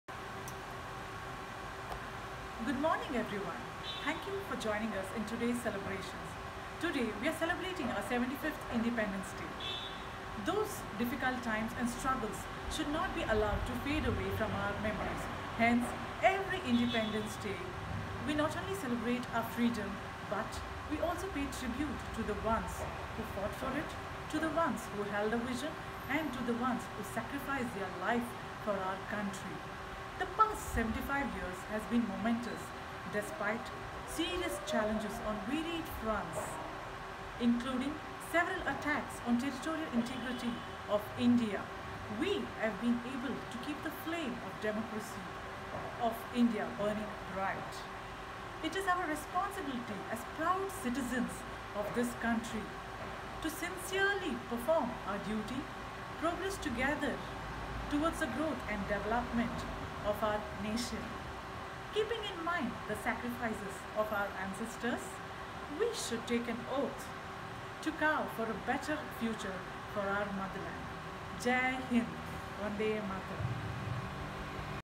Mount Litera Zee School, Whitefield 75th Independence Day Celebration
Independence Day speech by Principal Ma'am.